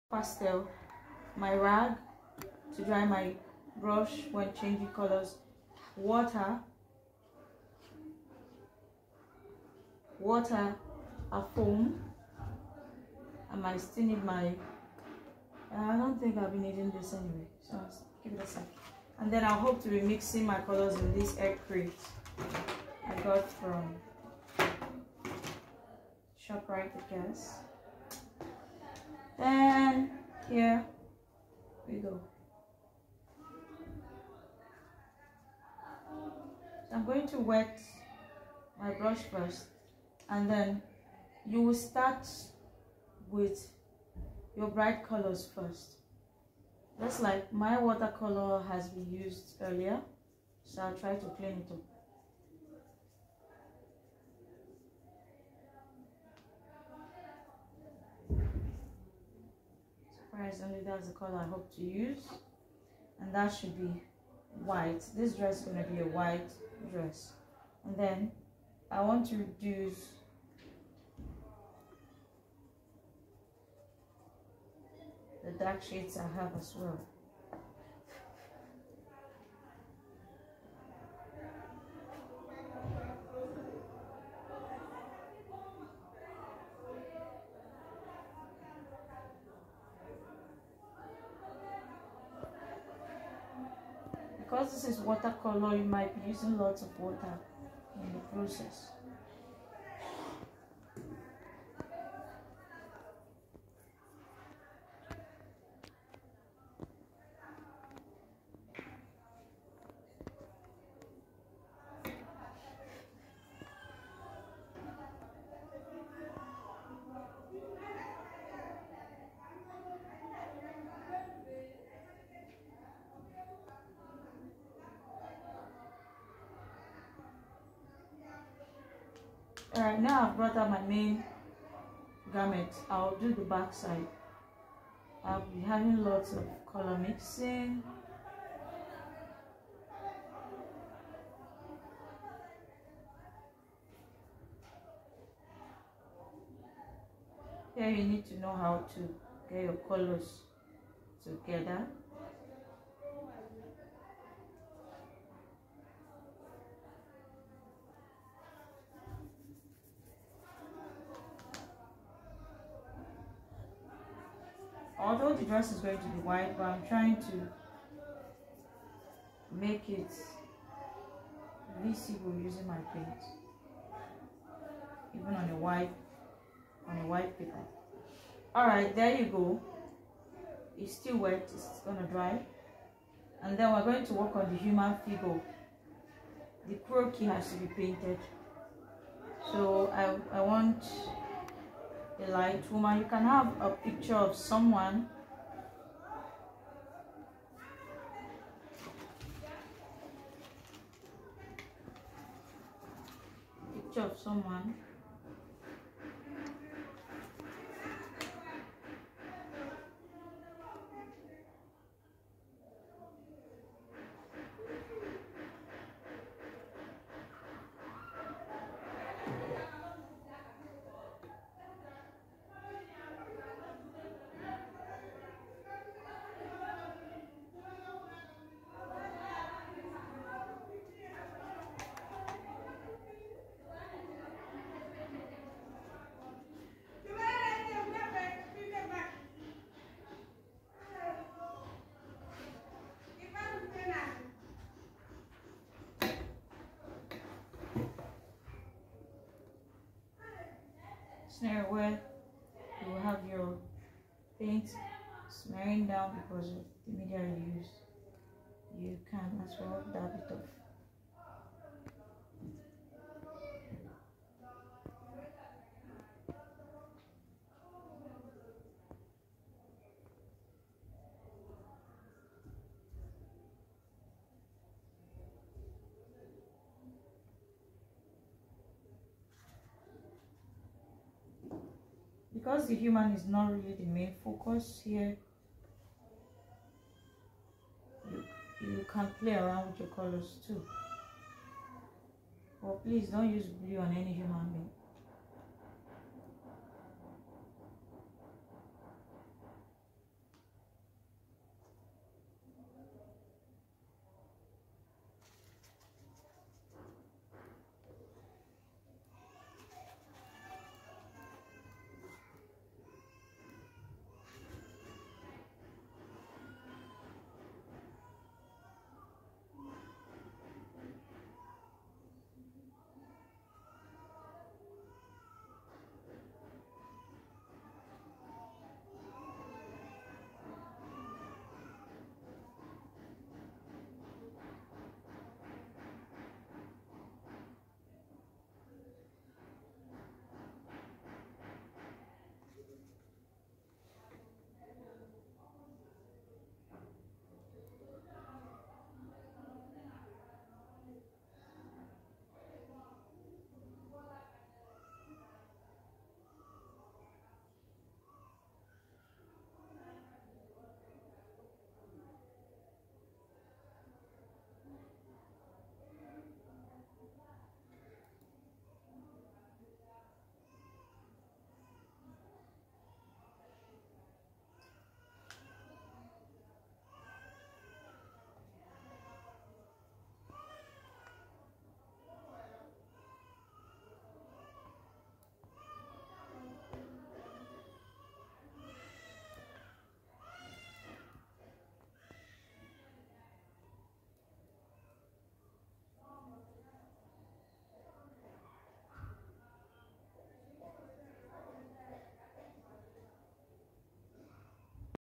DIY Fashion illustration
Here it is time to make our earlier drawing colourful... observe closely the tools we are using, they could be easily sourced from your local art market..